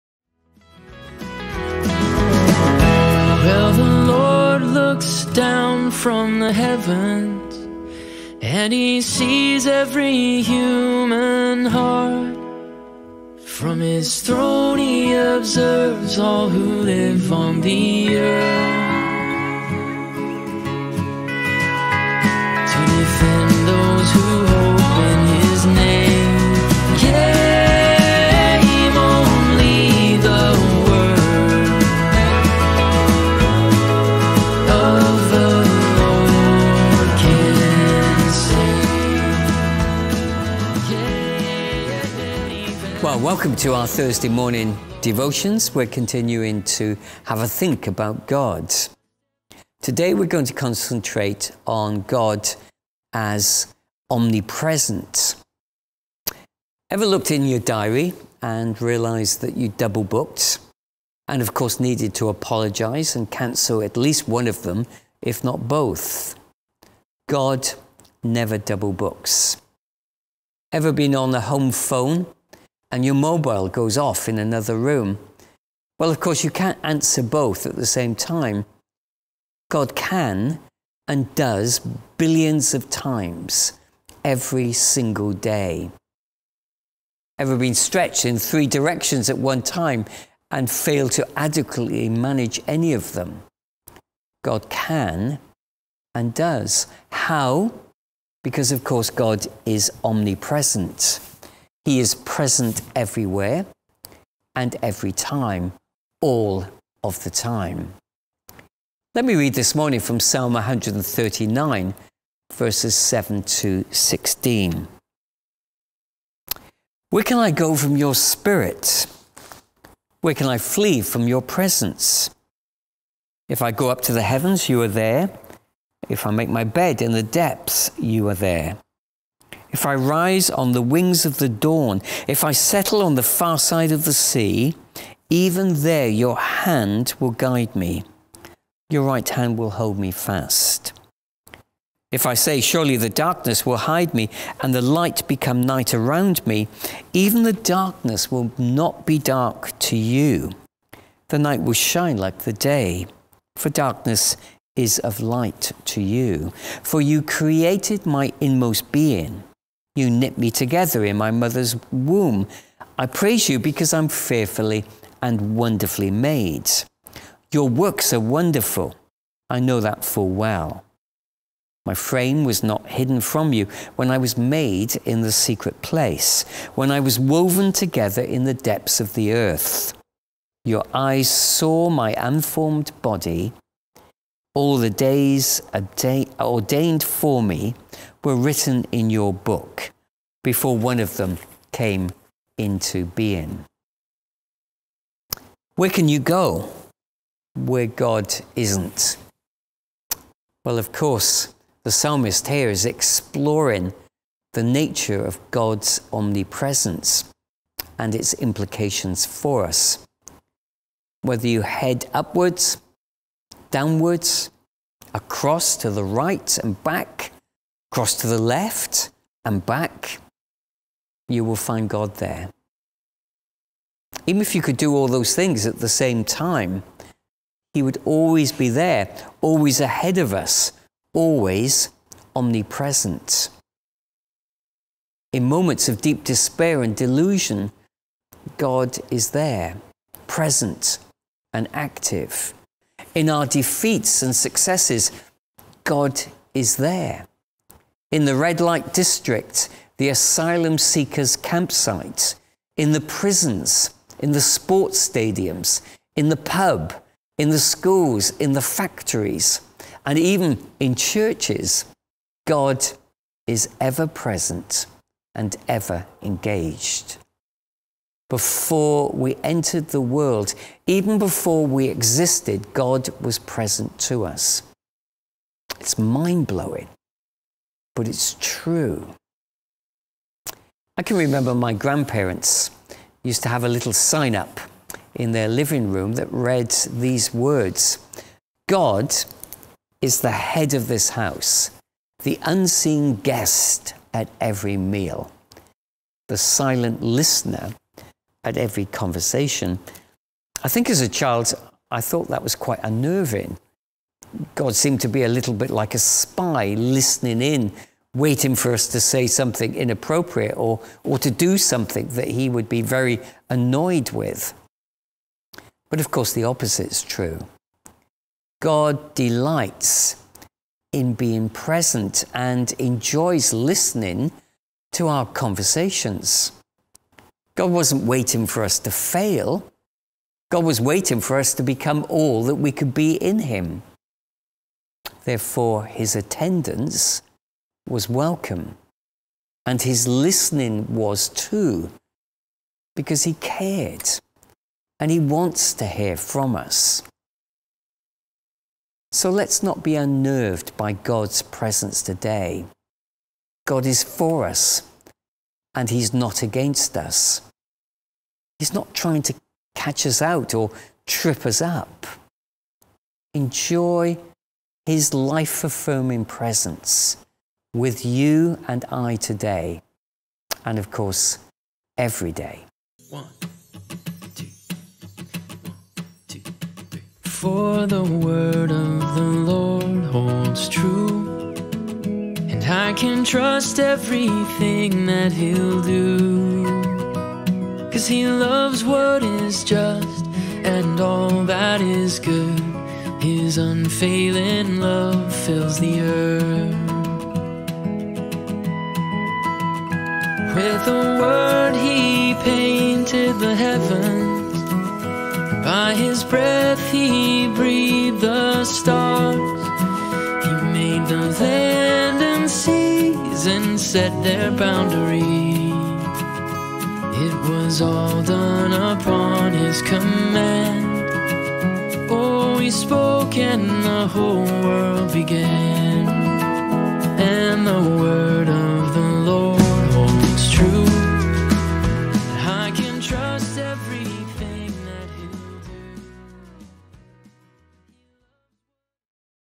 Daily Bible Readings – Psalm 139:7-16 – Thursday 9th June 2022
God the Omnipresent
Ever looked at your diary, realised you have double booked, and had to apologise and cancel one of them?
God never double-books.
Ever been on the home phone and your mobile has gone off in another room? You can’t answer both at the same time.
God can and does billions of times over every single day.
Ever been stretched in three directions at one time and failed to adequately manage any of them?
God can and does.

How? Because God is omnipresent. He is present everywhere and every time - all of the time.